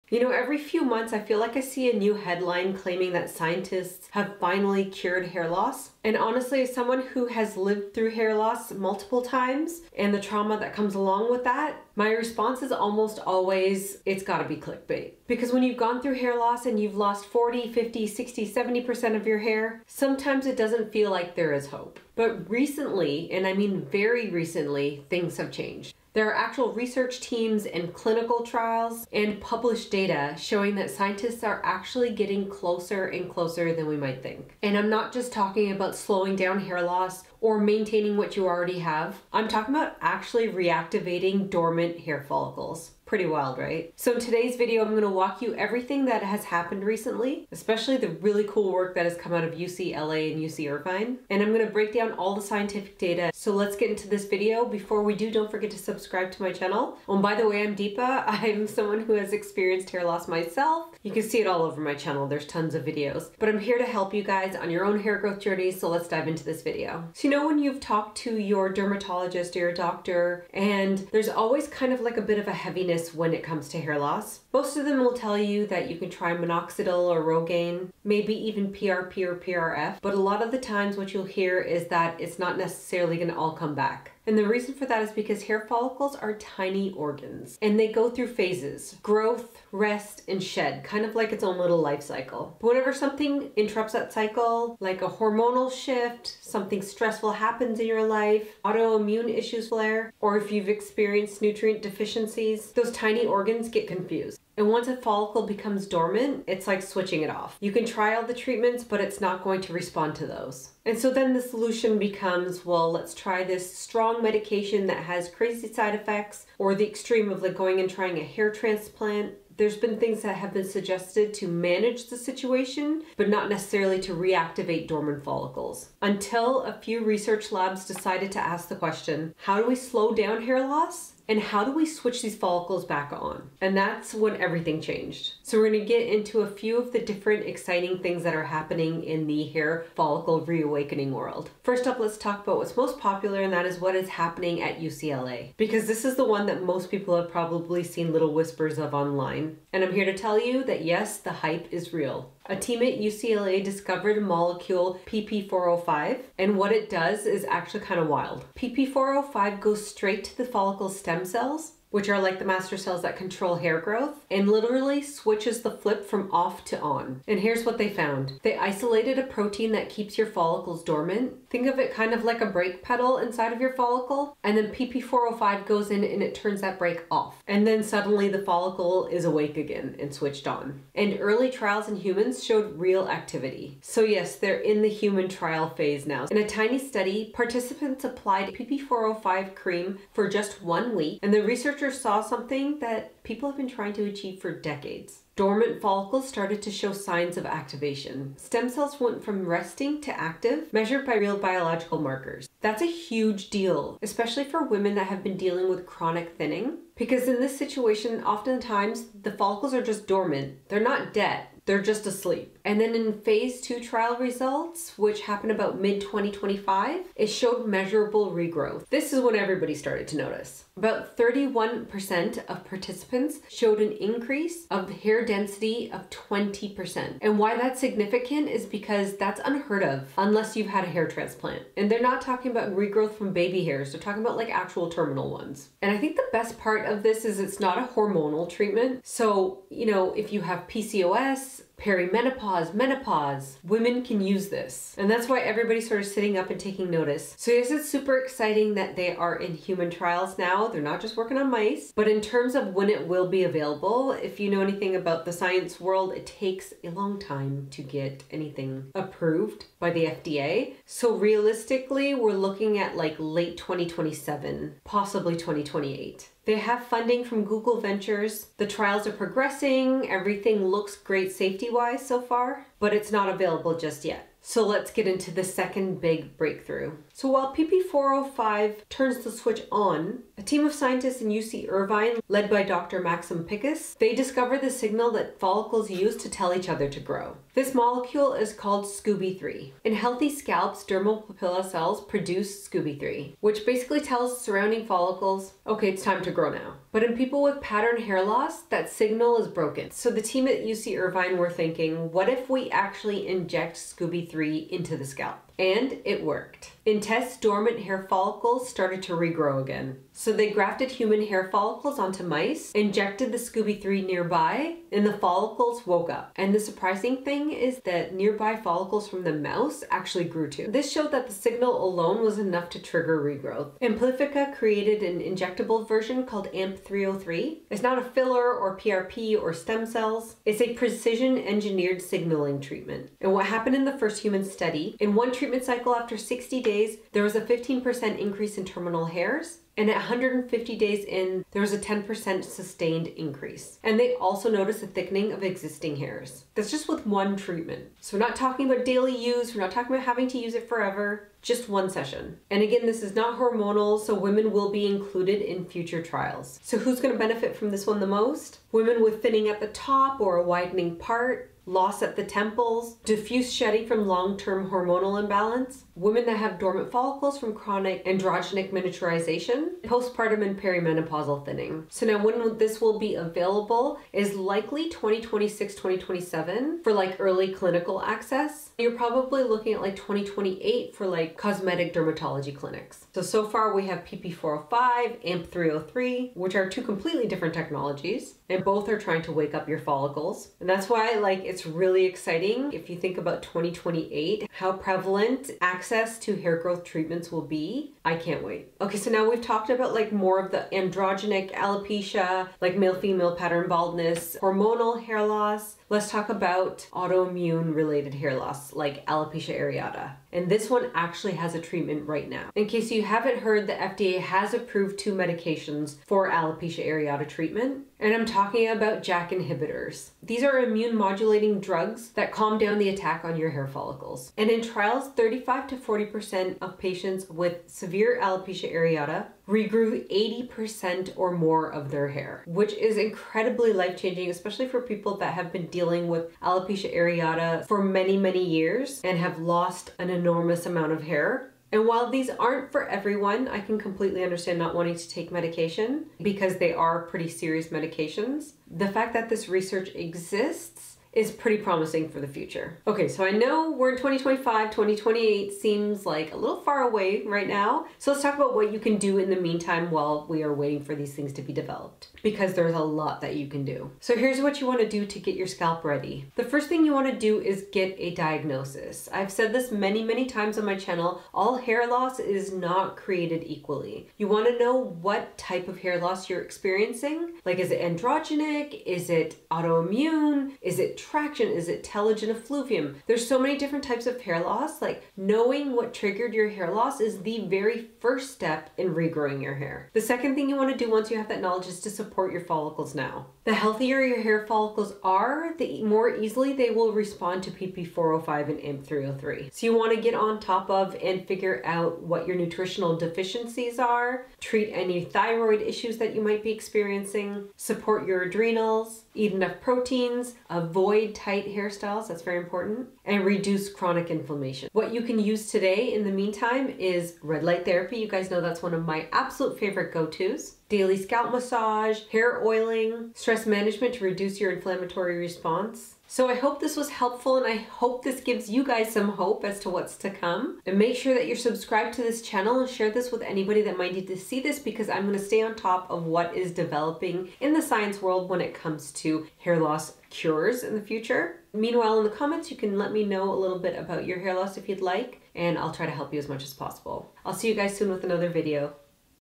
Has hair loss been cured for good
In today's video we're going over some of the amazing science coming out of UCLA, UC Irvine, and several biotech companies! This is definitely the most promising we’ve ever seen especially for women with genetic thinning, postpartum hair loss, autoimmune hair loss, and perimenopausal shedding.

00:00 Intro
01:07 Understanding Hair Follicles and Dormancy
02:21 Breakthrough at UCLA: PP 405 Molecule
04:37 UC Irvine's Discovery: Scooby 3 Molecule
06:45 Current Treatments for Autoimmune Hair Loss
07:32 Preparing for Future Treatments

AMAZON Shop for all my hair growth recs:

FREE Resources:
AIP Reintroduction Workbook: